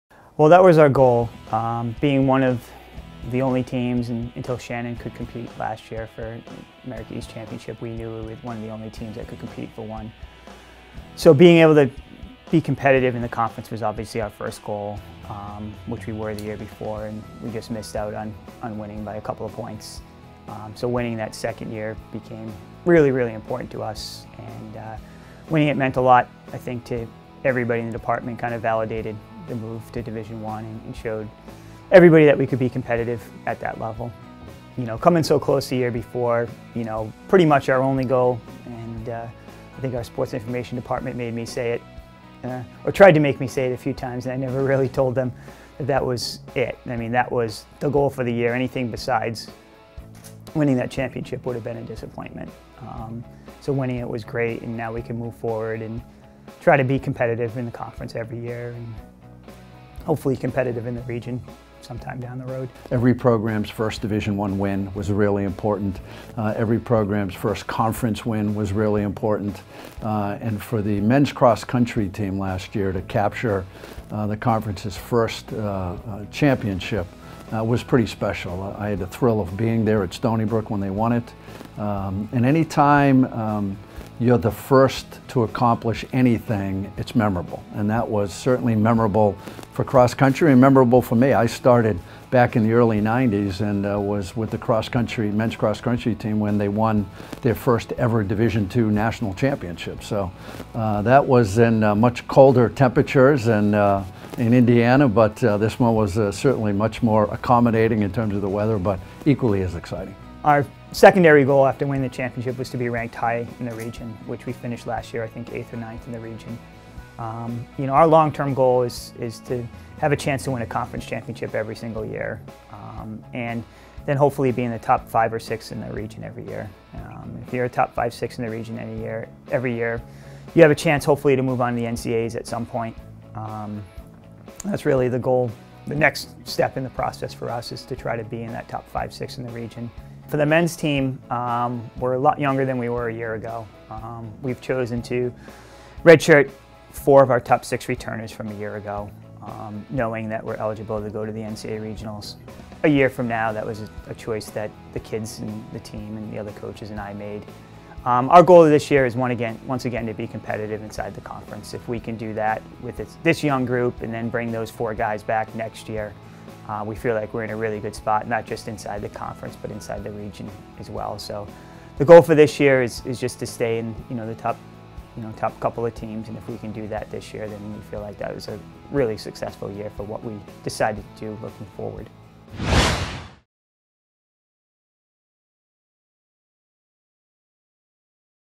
UMass Lowell Cross Country Striving for Sustained Success
Coach Gary Gardner talks about UMass Lowell's first Division I cross country championship and the team's goals for the future. Director of Athletics Dana Skinner adds his thoughts on the school's first America East championship team.